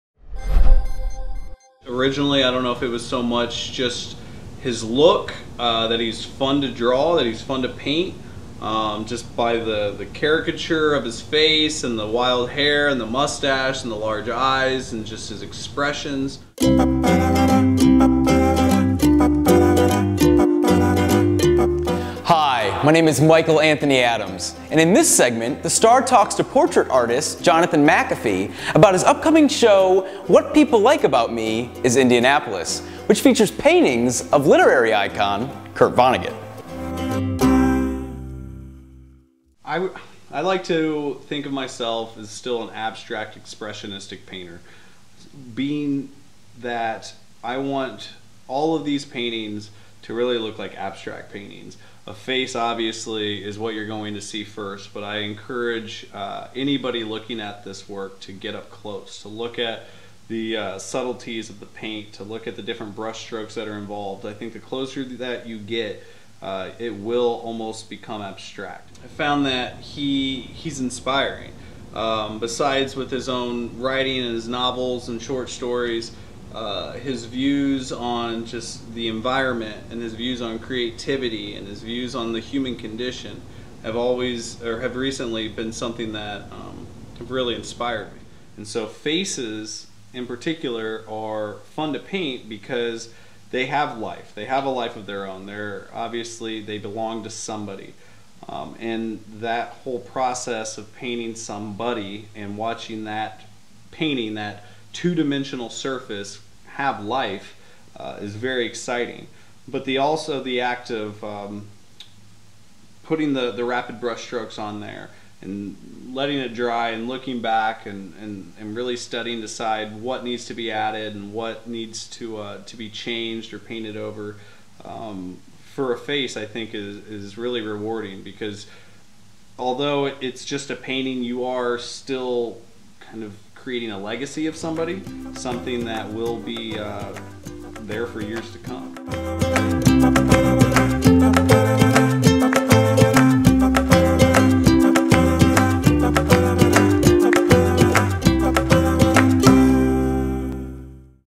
Face to Face with Jonathan McAfee
I caught up with artist Jonathan McAfee in the basement of his home in Garfield Park to talk about his new series "What People Like Me is Indianapolis," which features paintings of literary icon, Kurt Vonnegut